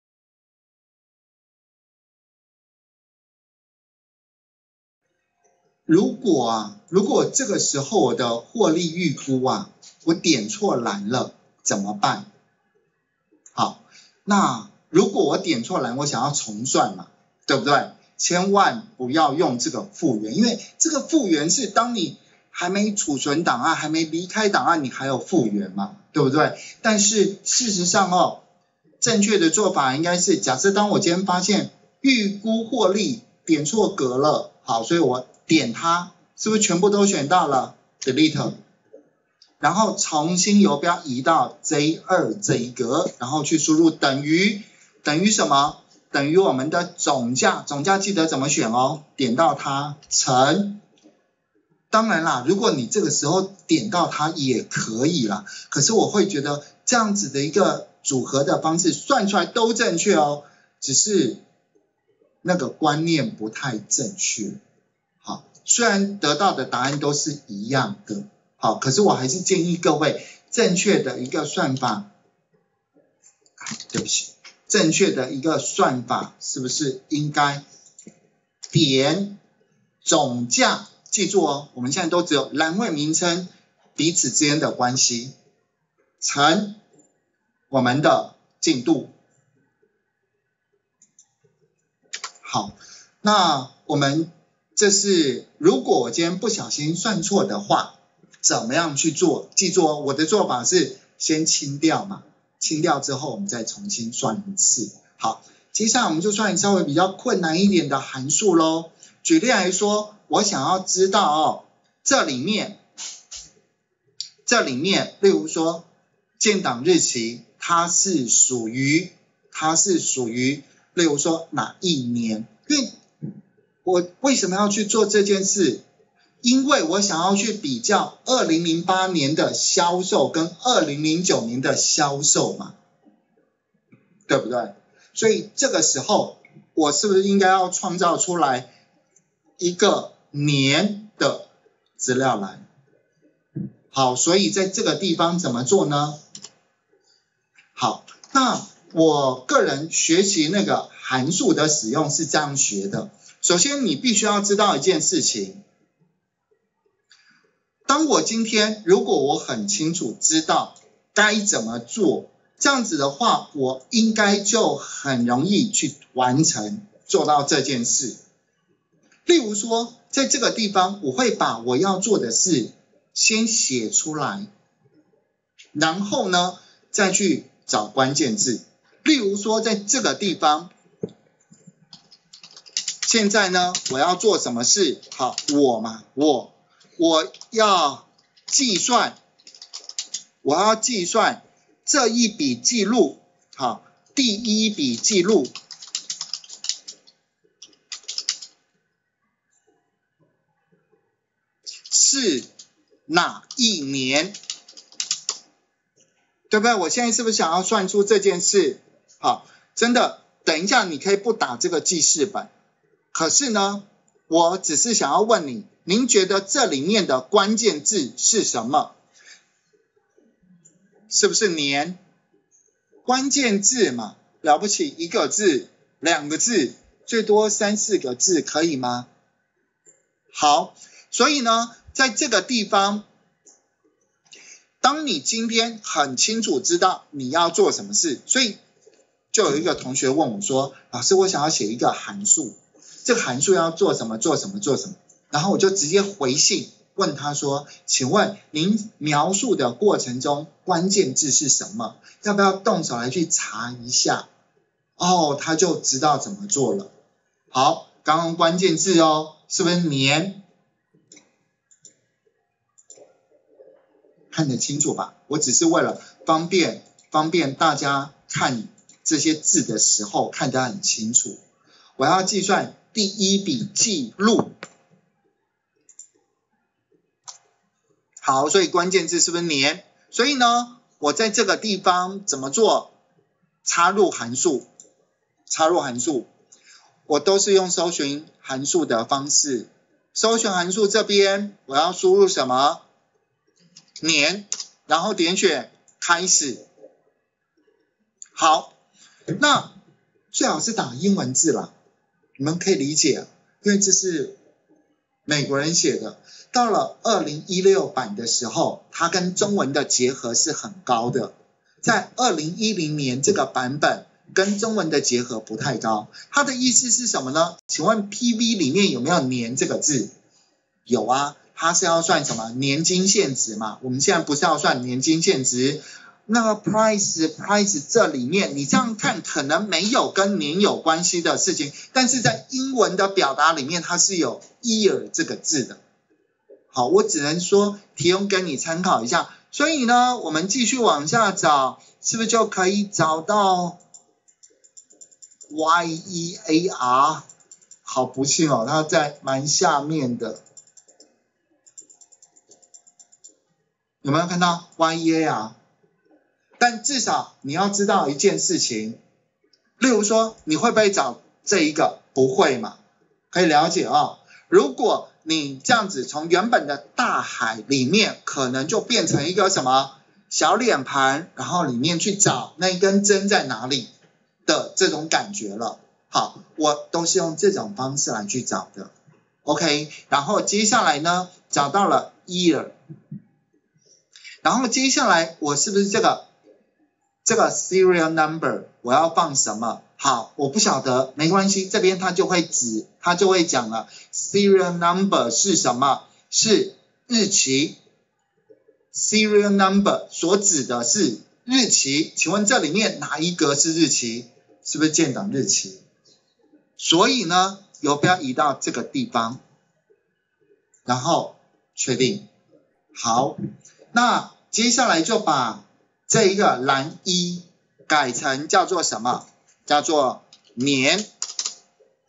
Excel 進階:09.搜尋函數找到適合的函數
在眾多函數中，如何找到自己要使用的函數？我發現大家最常做的事，就是問Google大神。在使用函數中，也有搜尋函數的功能，也可以快速找到函數哦！至少可以從數千個函數篩選掉只剩幾個函數。